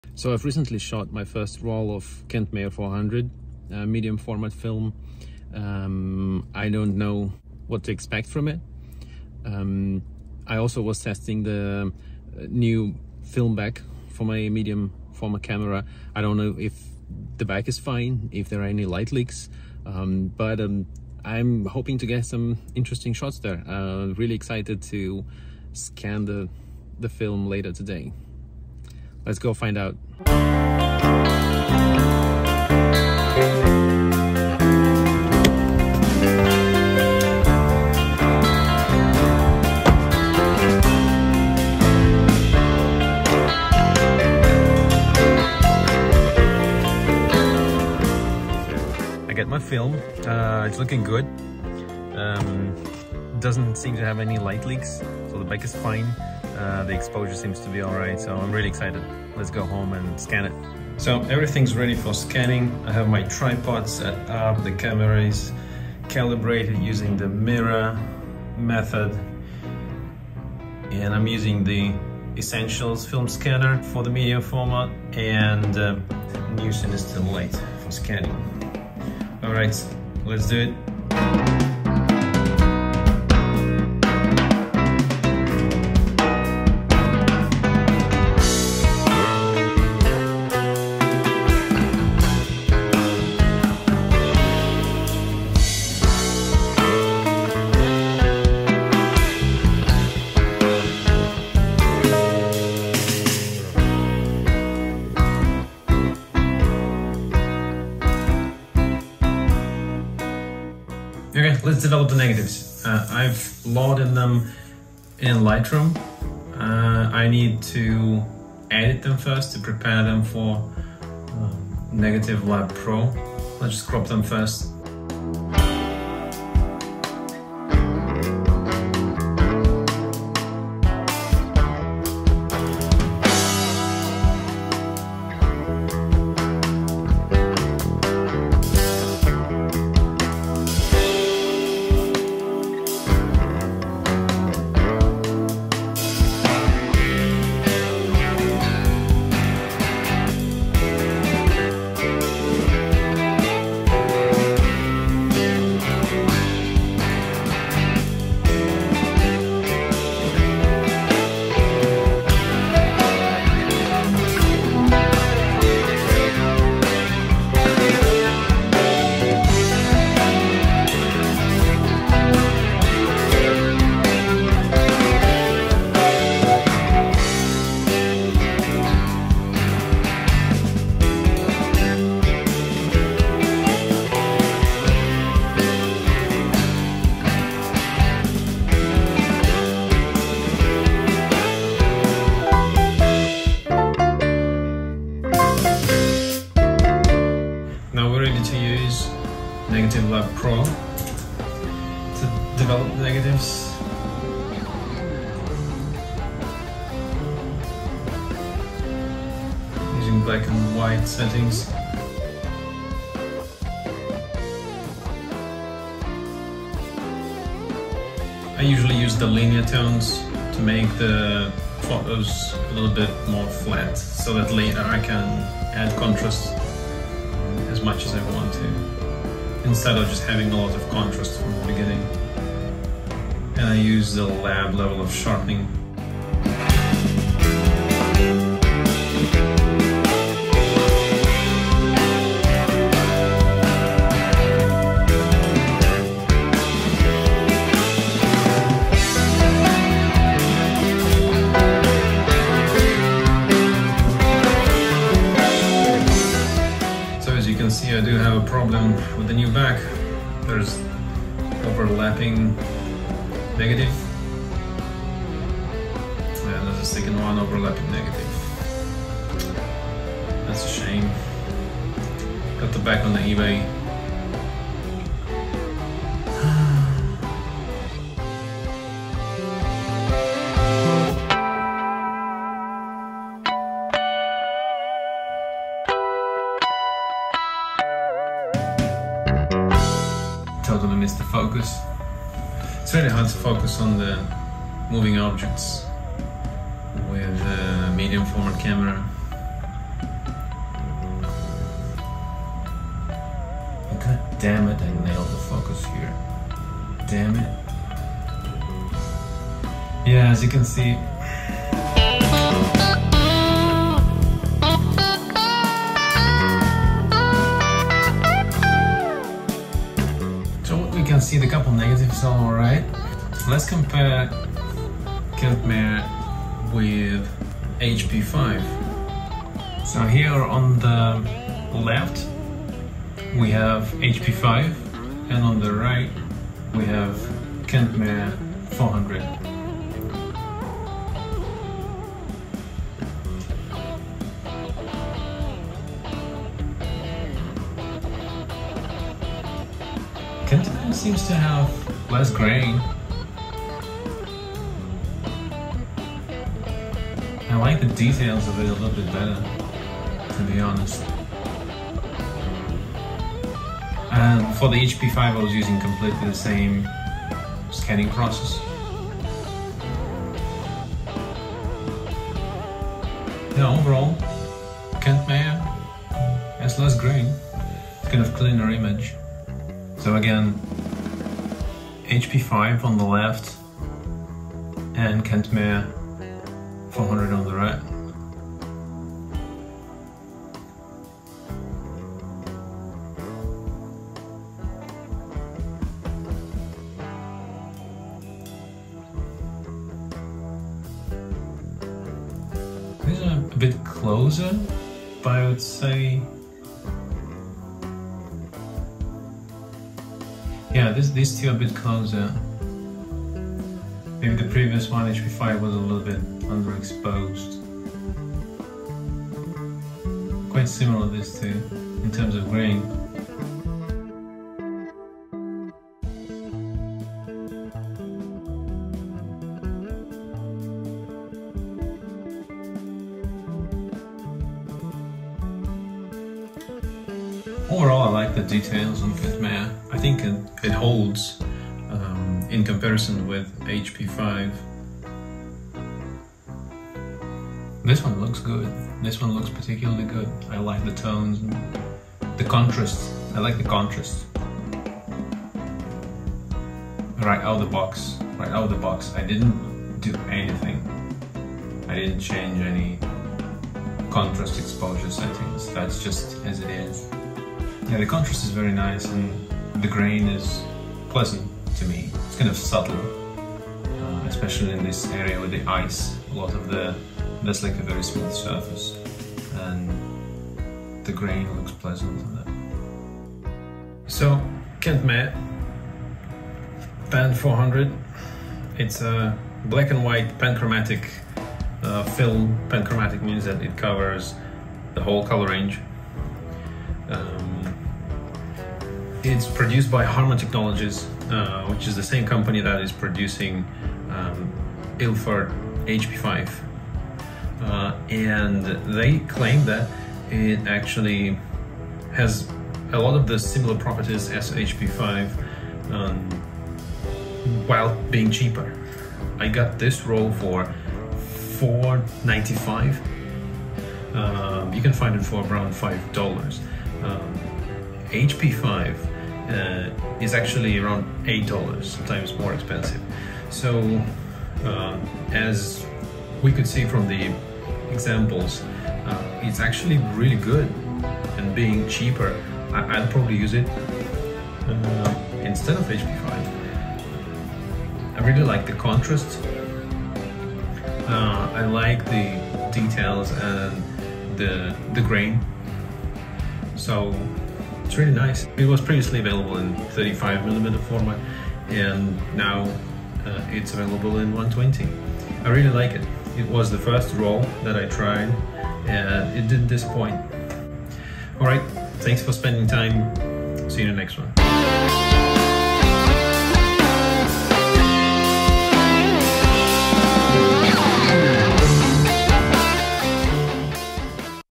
Kentmere 400 Test Roll – it is IMPRESSIVE!
I've shot my first roll of 120 film Kentmere Pan 400. I scan it and review the results comparing to Ilford HP5 Plus. Can Kentmere be a cheaper replacement for classic HP5?